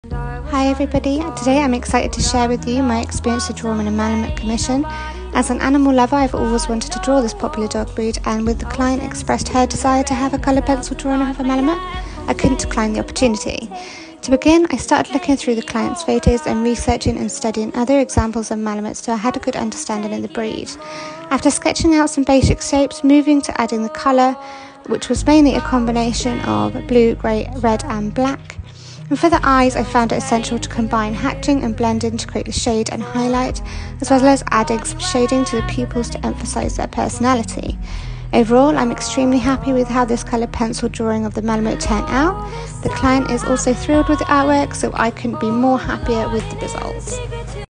Drawing a Malamute
This was a custom commission for Christmas of a malamute thought I would share